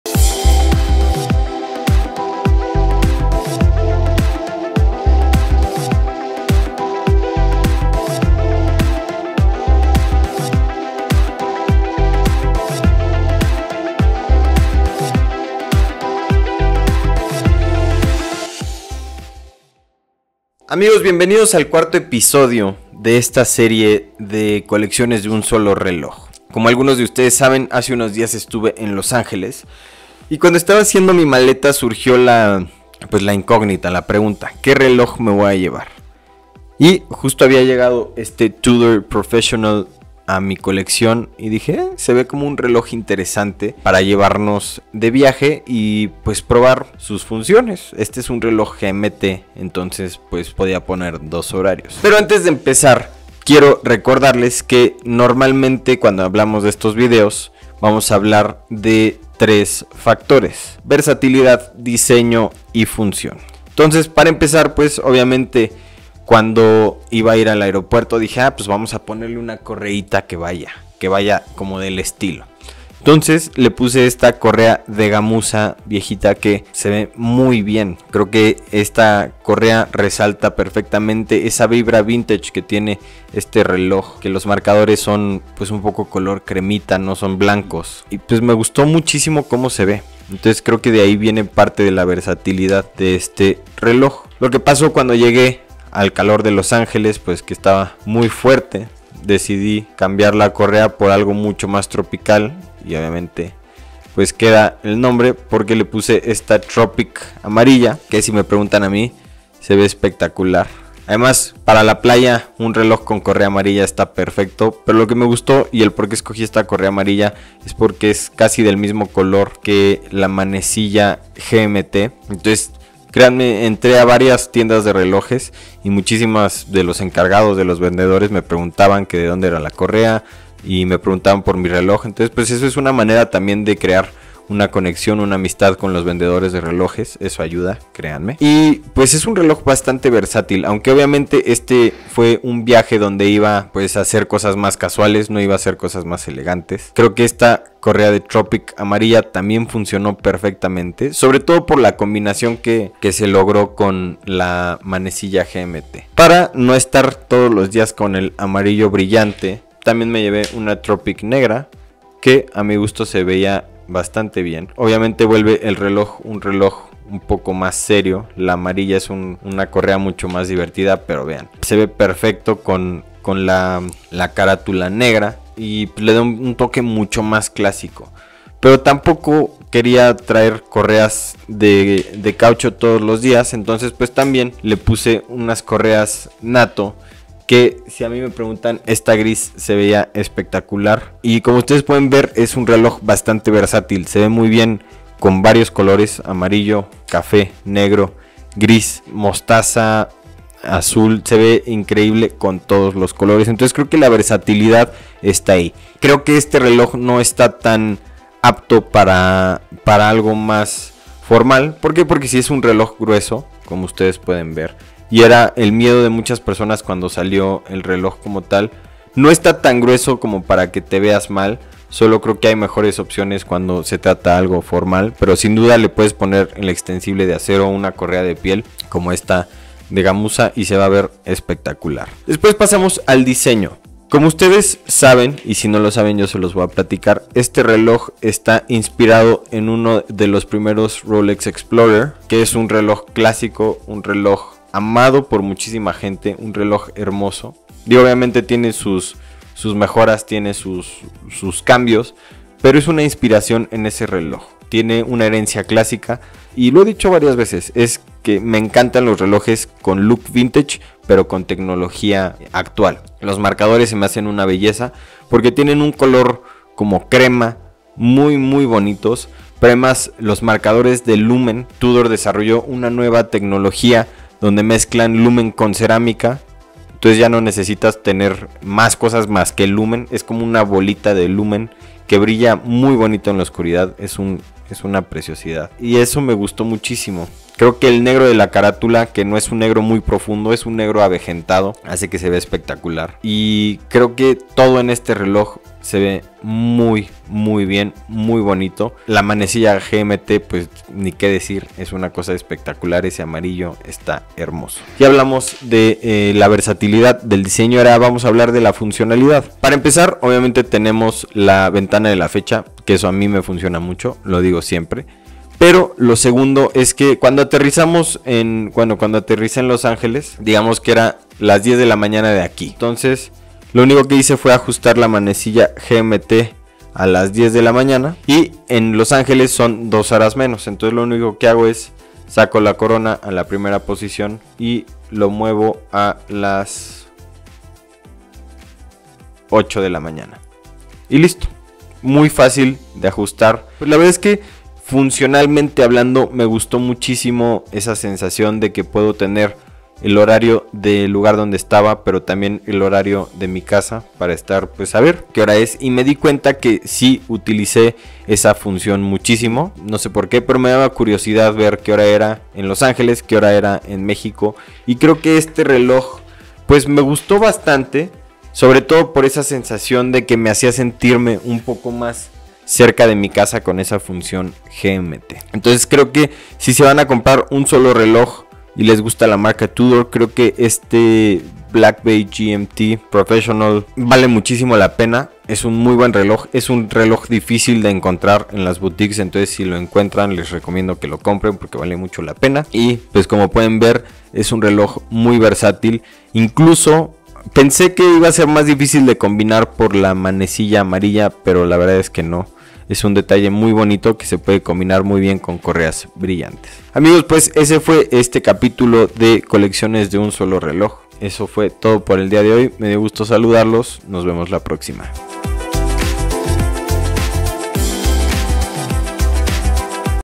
Si Solo Pudieras Tener un Reloj... Tudor Black Bay Pro. Ep 4 Mi Yorch Style
Te invito a acompañarme en este camino, hablemos de relojes, zapatos, estilo y ¡mucho más!